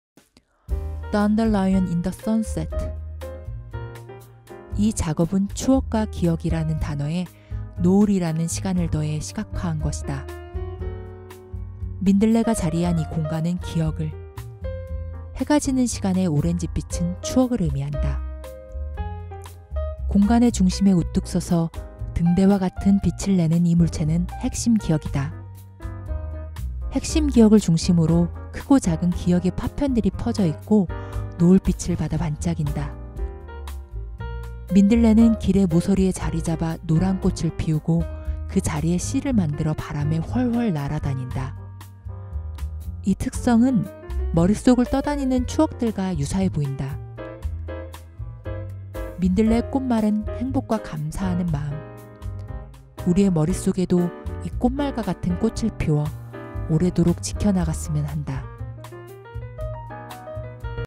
Dandelion in the sunset l 설치예술 l 온라인 전시회 l
Dandelion in the sunset

이 작업은 추억과 기억이라는 단어에 노을이라는 시간을 더해 시각화 한 것이다. 민들레가 자리한 이 공간은 ‘기억’을, 해가 지는 시간의 오렌지빛은 추억을 의미한다.

공간의 중심에 우뚝 서서, 등대와 같은 빛을 내는 이 물체는 핵심 기억이다. 핵심 기억을 중심으로 크고 작은 기억의 파편들이 퍼져있고, 노을빛을 받아 반짝인다.

민들레는 길의 모서리에 자리 잡아 노란 꽃을 피우고, 그 자리에 씨를 만들어 바람에 훨훨 날아다닌다. 이 특성은 머릿속을 떠다니는 추억들과 유사해 보인다.

민들레의 꽃말은 행복과 감사하는 마음.
우리의 머릿속에도 이 꽃말과 같은 꽃을 피워, 오래도록 지켜나갔으면 한다.